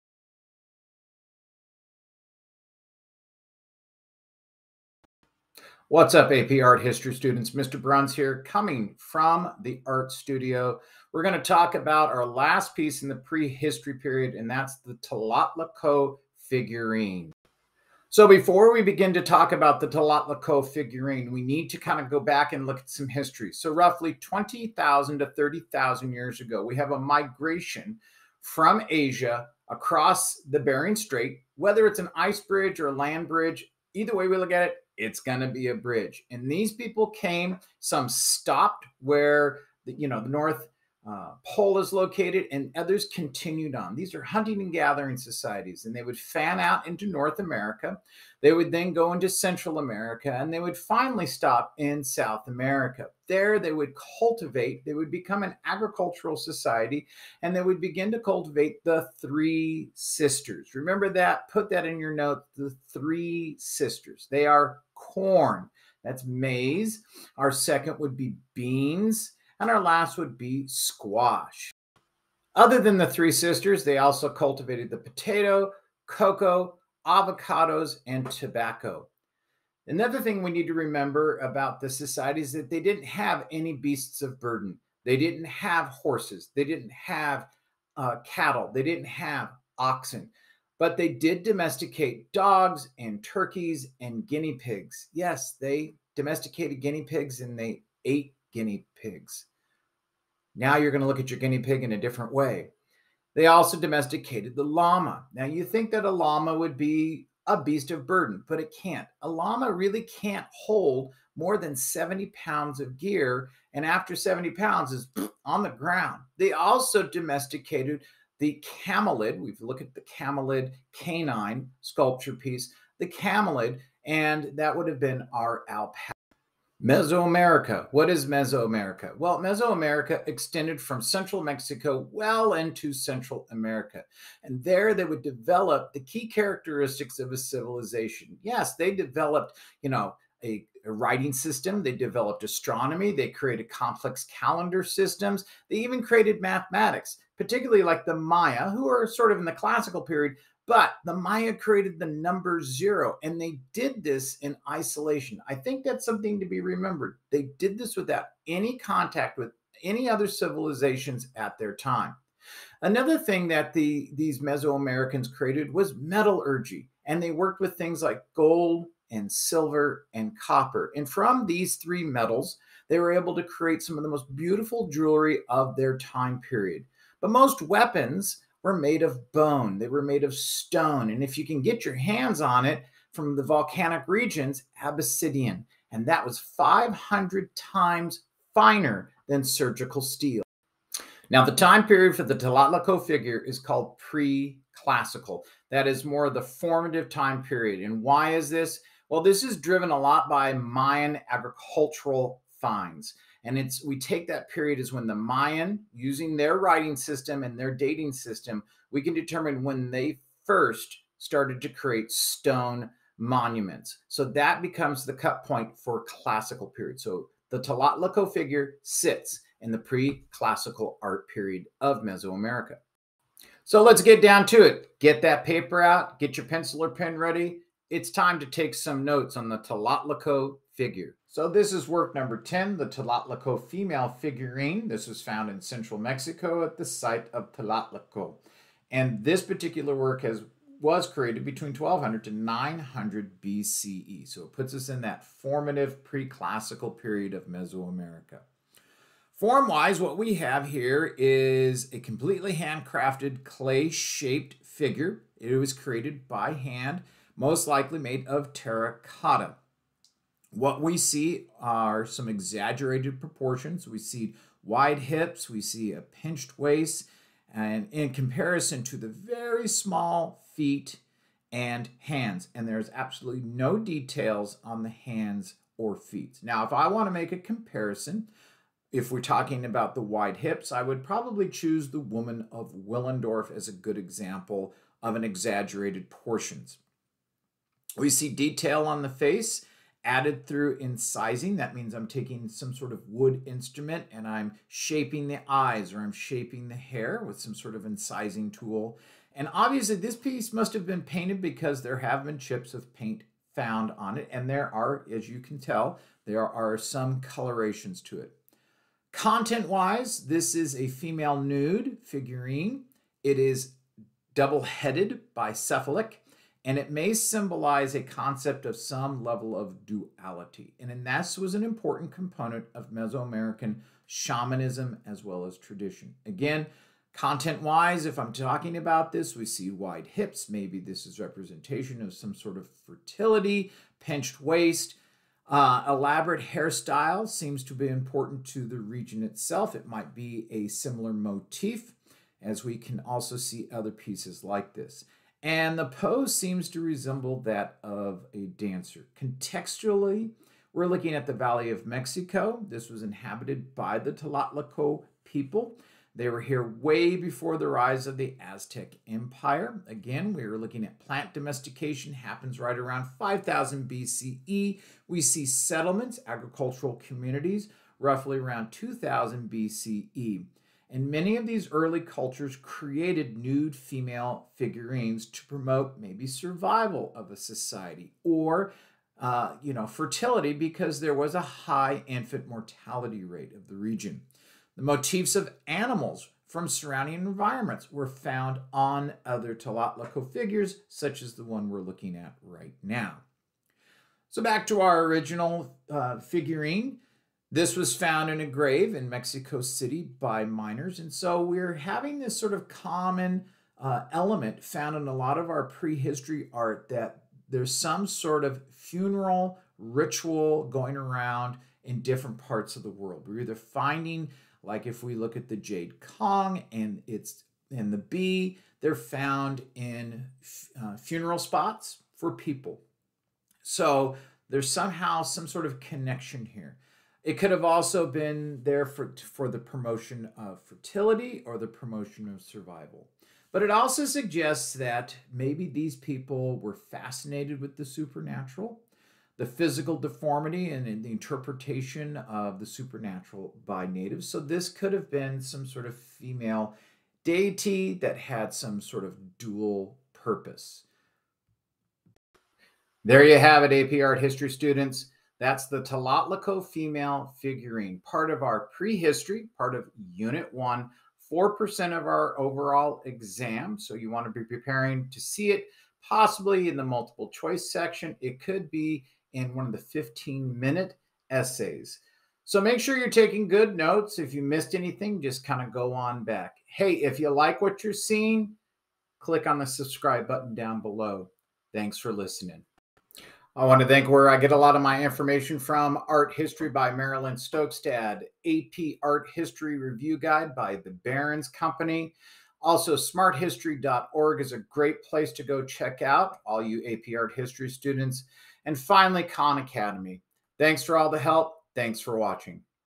AP Art History Unit 1 Mesoamerica
Lecture for AP Art History Unit 1 Prehistory art in Mesoamerica with a focus on the Tlatlico Female Figurine